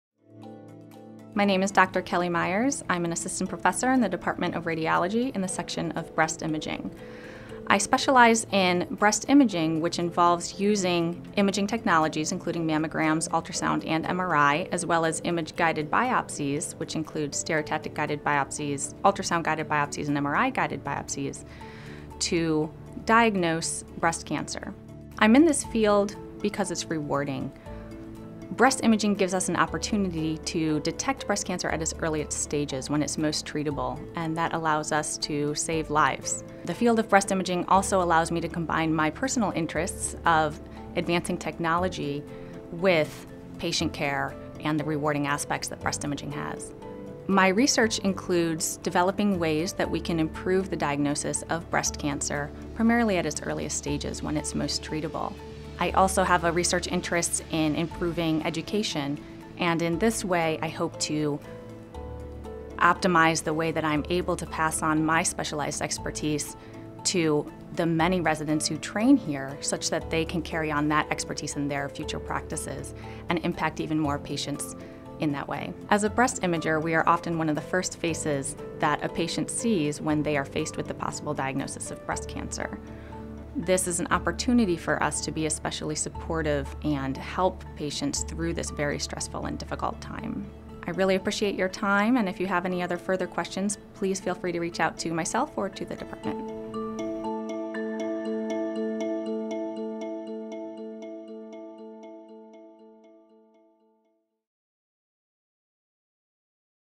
Dr. Kelly Myers | Breast Imaging Radiology
Kelly Suzanne Myers, M.D. is an assistant professor of Radiology and Radiological Science with the specialization in Breast Imaging.